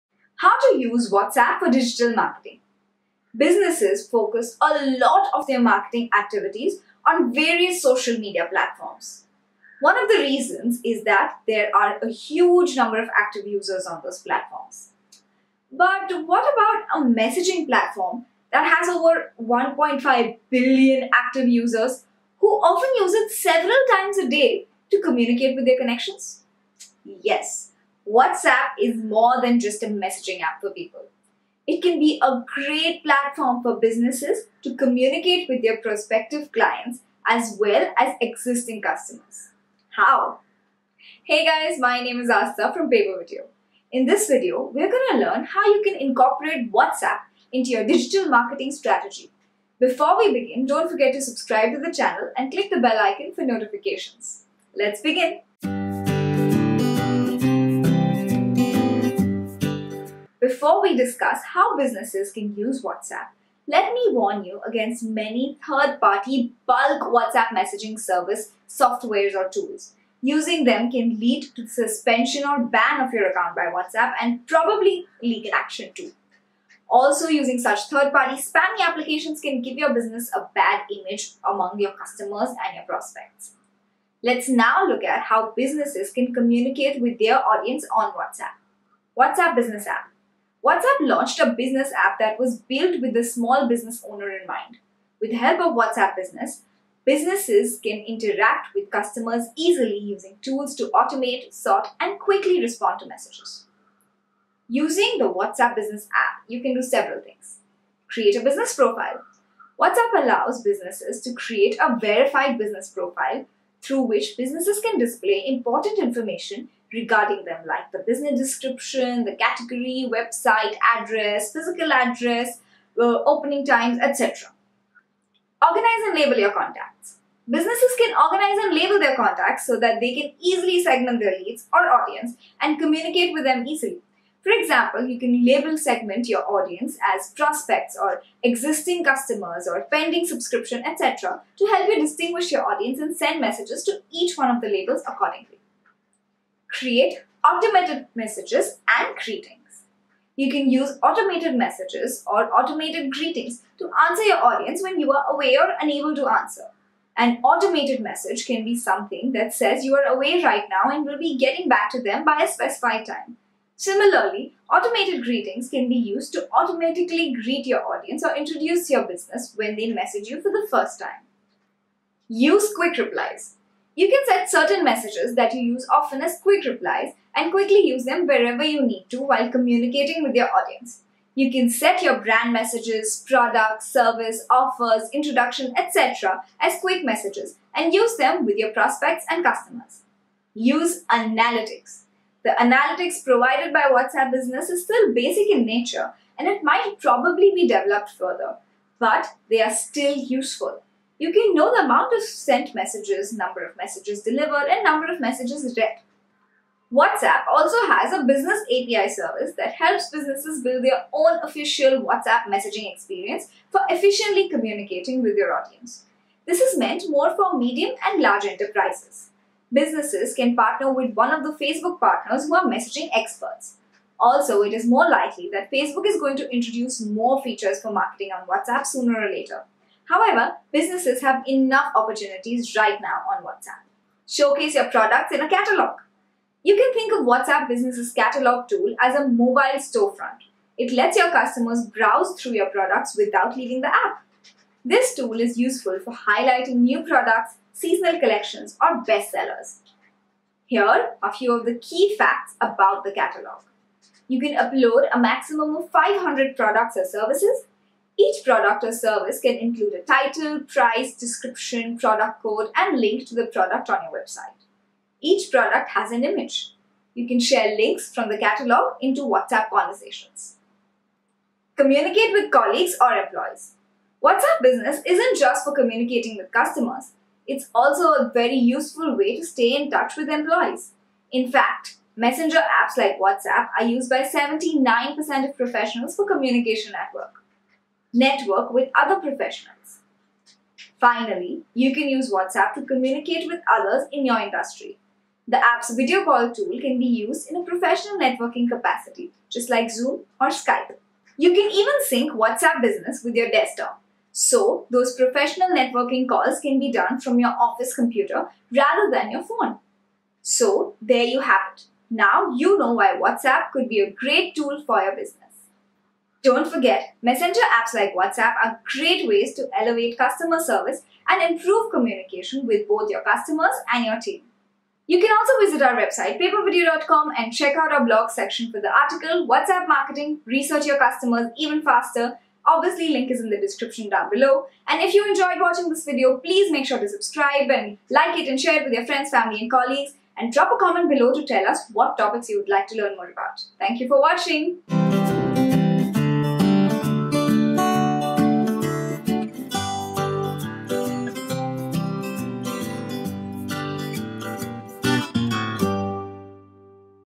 How to use Whatsapp for digital marketing
In this video, we will explain to you how to use Whatsapp for digital marketing for better business success. It will inform you about WhatsApp business for creating a business profile to label your contact, automated message, quick replies, WhatsApp analytics, product catalog, and much more.

Overview of video:

The video will show you how to do digital marketing with WhatsApp business. We will start with creating a business profile after some tips for organizing and labeling your contacts and how to use automated messaging and quick replies for marketing.

After that, we'll inform you of the importance of analytics in digital marketing success and the importance of showcasing your product in the WhatsApp catalog. At last, it will also explain the importance of networking with other professionals.

Video Content:

0:00 Introduction
1:47 Create a Business Profile
2:05 Organize and label Your Contact
2:26 Create Automated Messages and greetings
2:52 Use Quick Replies
3:10 Use analytics
3:54 Showcase your products in a catalog
4:48 Network with other professionals